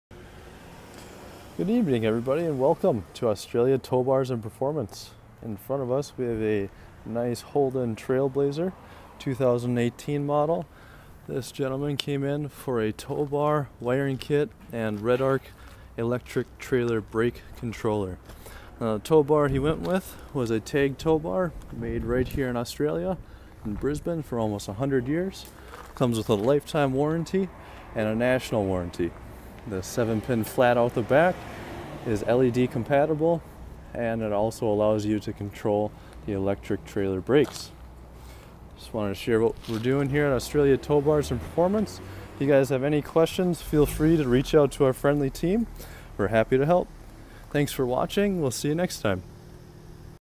2018 Holden Trailblazer Supply and Fit TAG Towbar Wiring kit & Redarc Electric Trailer Brakes
2018 Holden Trailblazer: Supply and Fit TAG Towbar Wiring kit & Redarc Electric Trailer Brakes.

Australia Towbars & Performance offers diagnostics, installation & coding in Maroochydore on the Sunshine Coast QLD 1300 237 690 & Australia wide